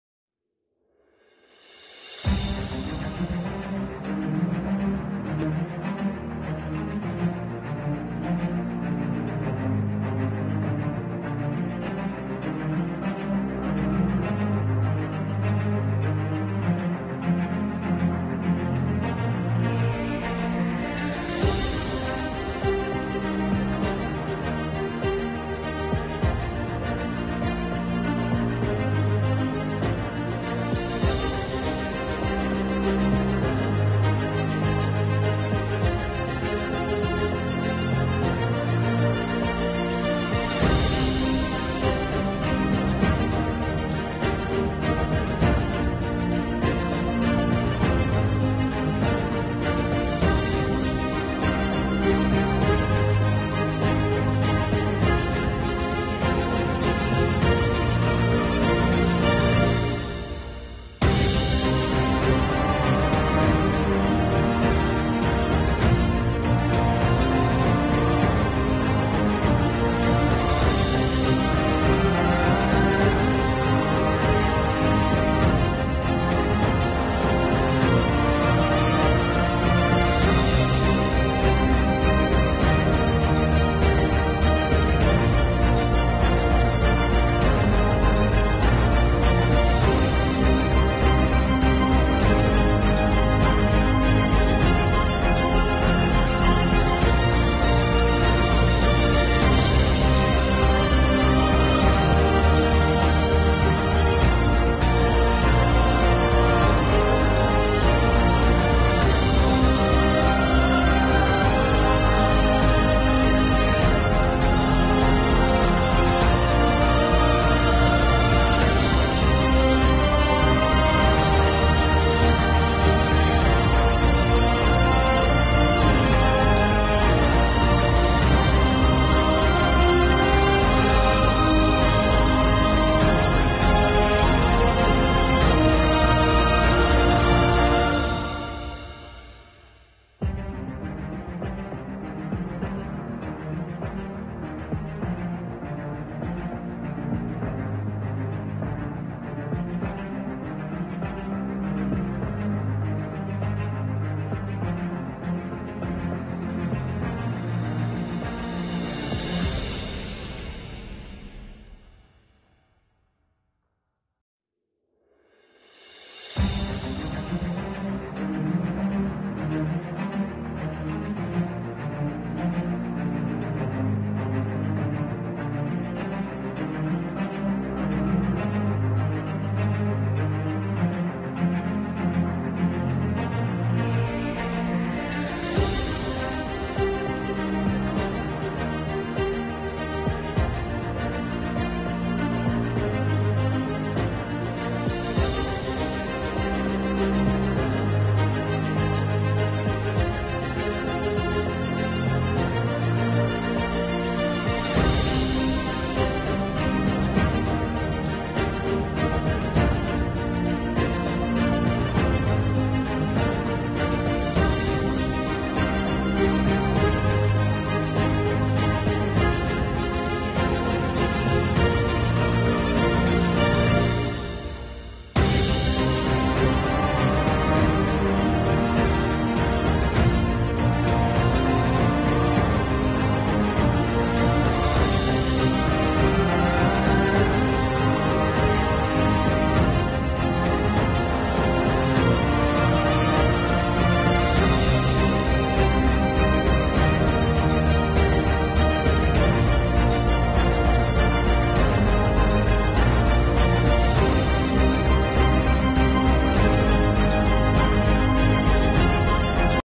Epone, Elisabethville, bout du monde
Depuis la salle du bout du monde jusqu'au rond point de l'A13 à Epone
Vitesse X2